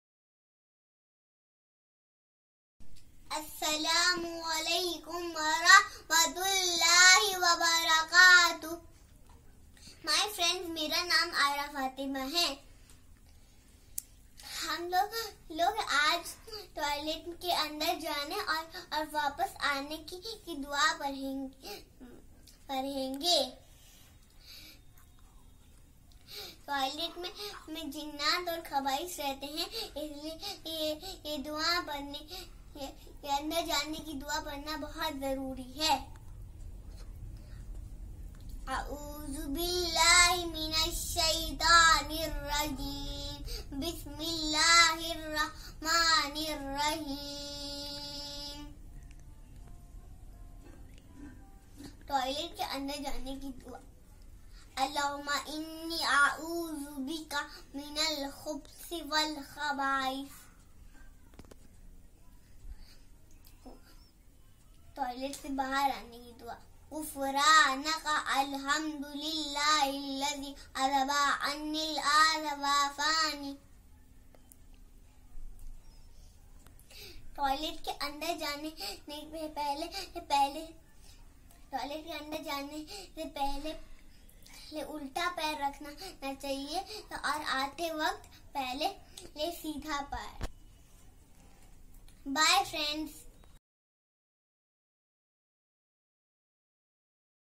Dua to say before going to washroom and after coming back|Adaab#dua
Learn with Ayerah Fatima proper way to go to washroom and coming back
Never forget to say these small Duas to be protected against Shaitan/Khabees